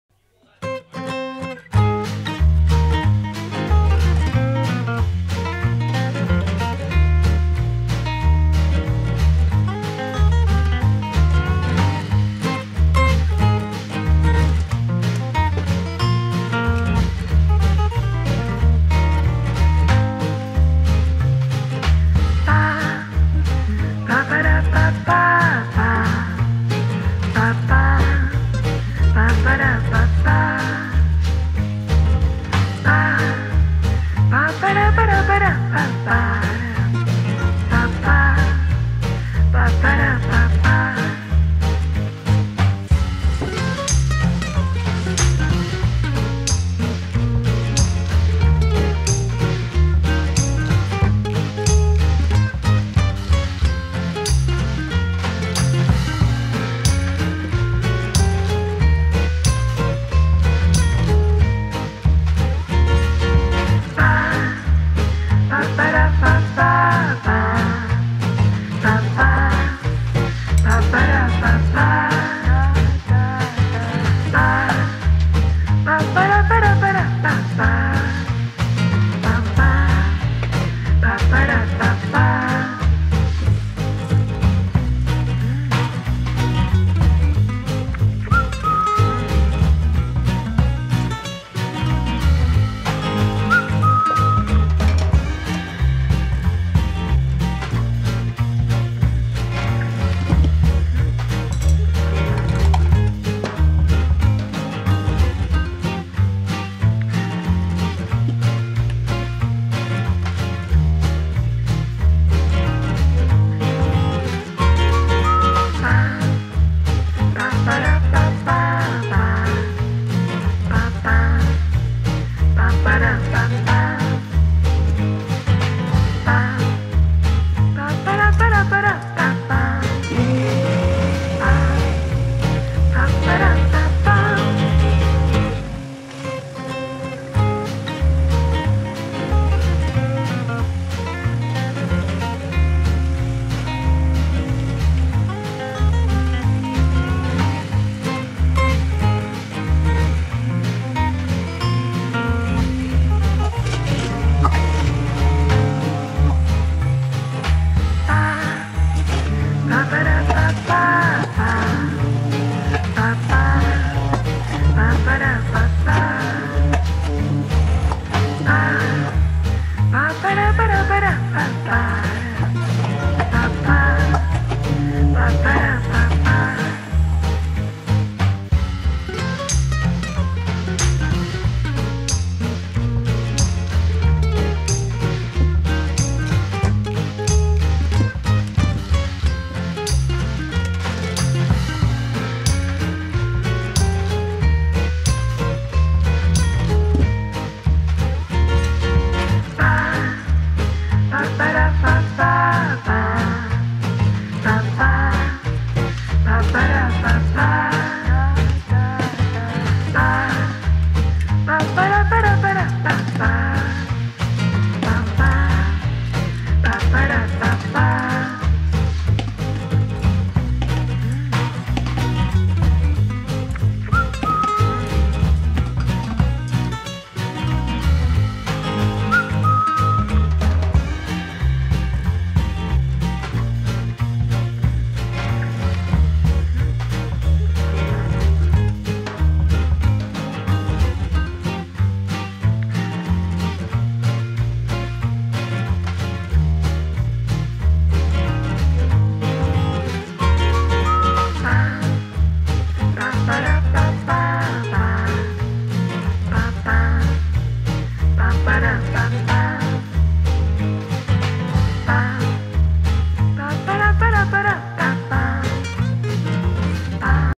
MAIONEZA RAPIDĂ și SIGURĂ DIN GĂLBENUȘURI FIERTE ȘI CRUDE 🥚🥣
Ingredients :
2 raw yolks,
2 hard-boiled yolks,
1 teaspoon of mustard,
1 pinch of salt,
 the juice of half a lemon or less.
300 ml sunflower oil.